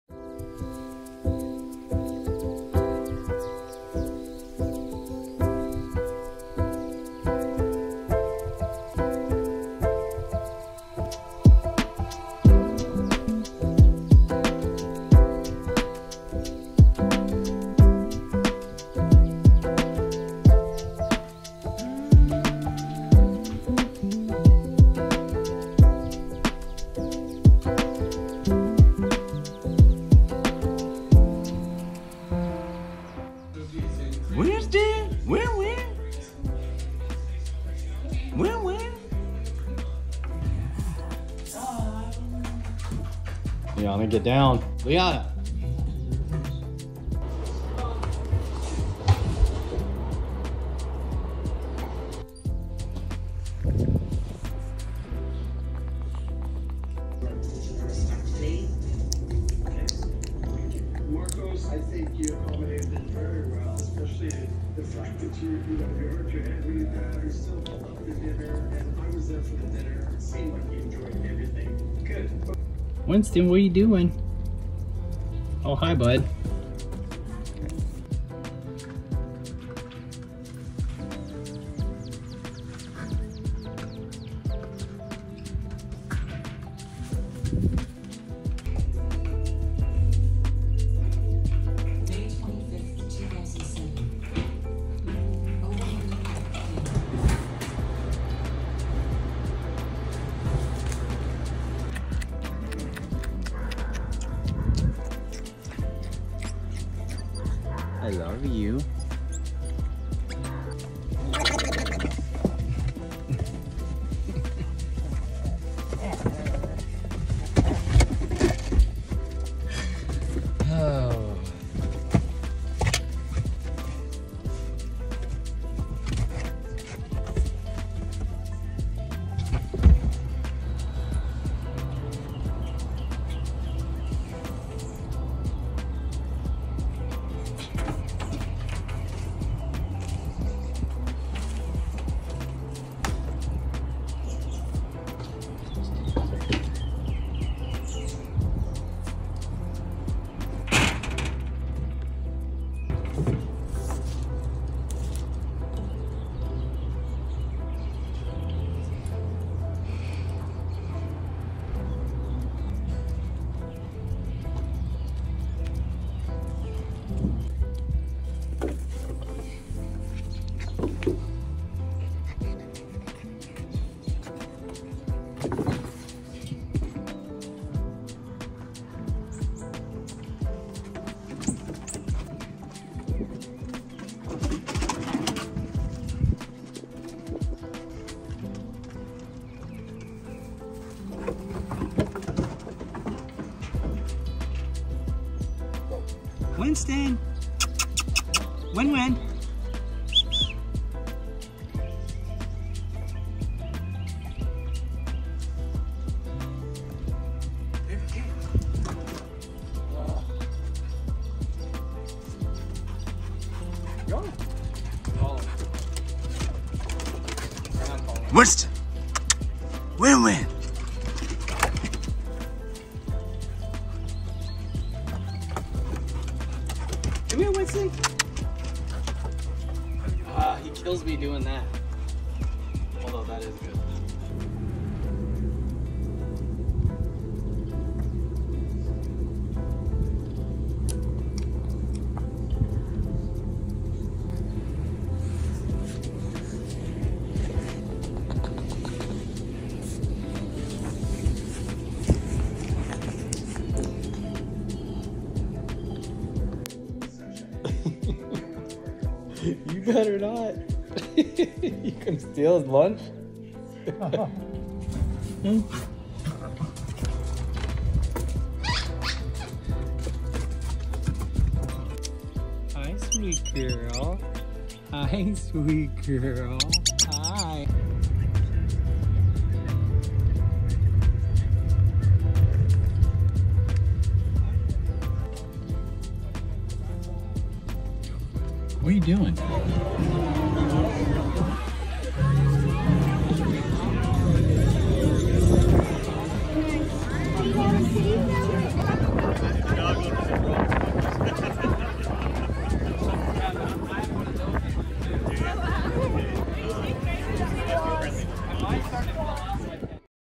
Man Has Adorable Bond With Baby SPIDER MONKEYS! (Pawsome Pets)
ID: 3494243

ONSCREEN CREDIT - @spidermonkeywinston

Watch the adorable bond formed by a man and two baby SPIDER MONKEYS! Michael Robinson, 42, has stuck up an unlikely friendship with Winston, ten months and two-year-old Leana. The dad-of-seven is currently hand-rearing the cheeky monkeys who require around the clock care. Michael, who owns animal rescue and rehabilitation centres, has shared videos illustrating his day to day life in the company of Winston and Leana. He also has a two-month-old lemur called Julian at his home in Tennessee, USA, along with a possum, two dogs and a tarantula. Michael said: “It is like having two toddlers running around the house. They require constant attention and enrichment otherwise they will injure themselves out of boredom and frustration. They can’t be left alone for longer than two hours as they need constant mental stimulation."